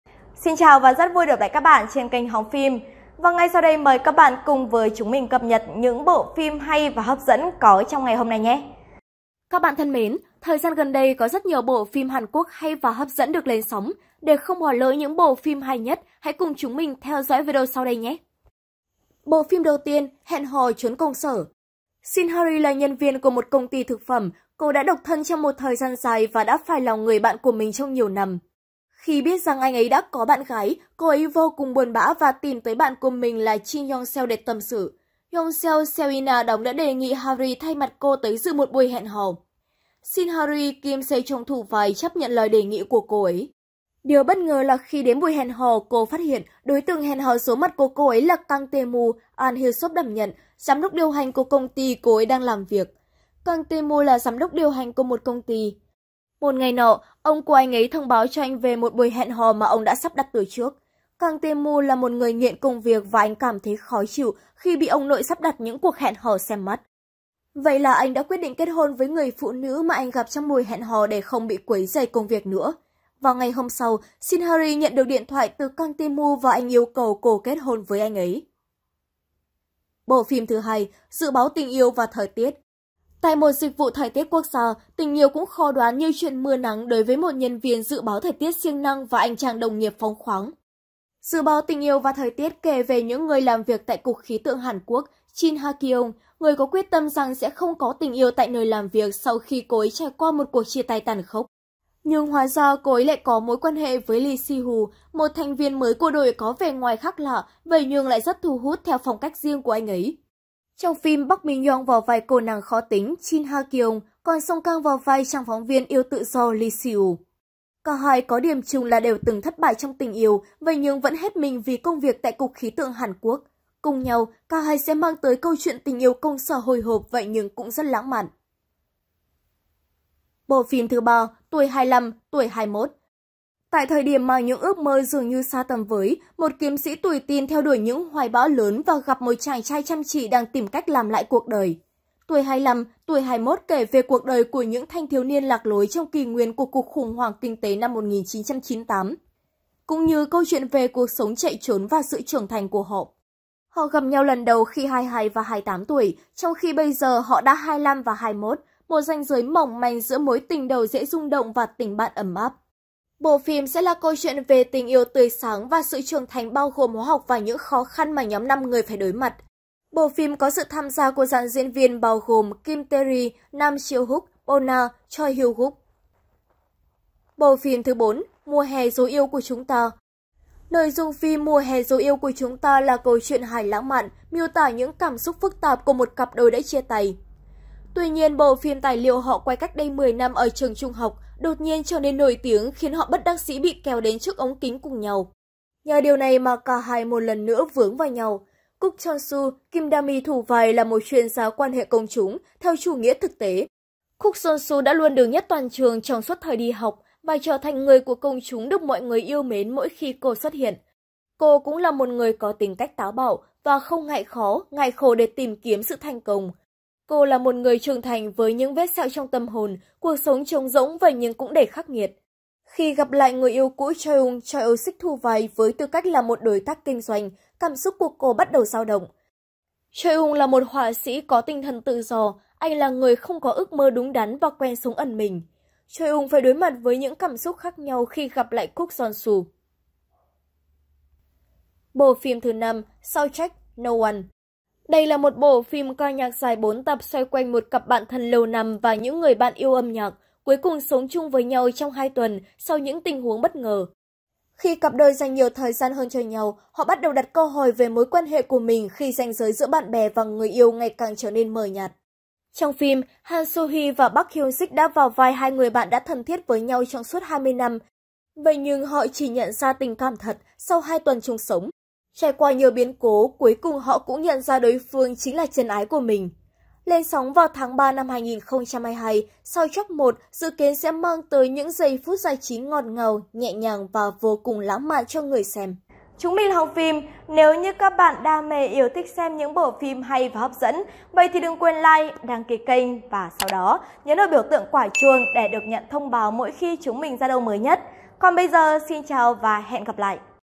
Top 5 phim tình cảm lãng mạn Hàn Quốc hay nhất lên đang lên sóng hiện nay|Hóng Phim
Các bạn thân mến thời gian gần đây có rất nhiều bộ phim Hàn Quốc hay và hấp dẫn được lên sóng. Để không bỏ lỡ những bộ phim hay nhất hãy cùng chúng mình theo dõi video sau đây nhé!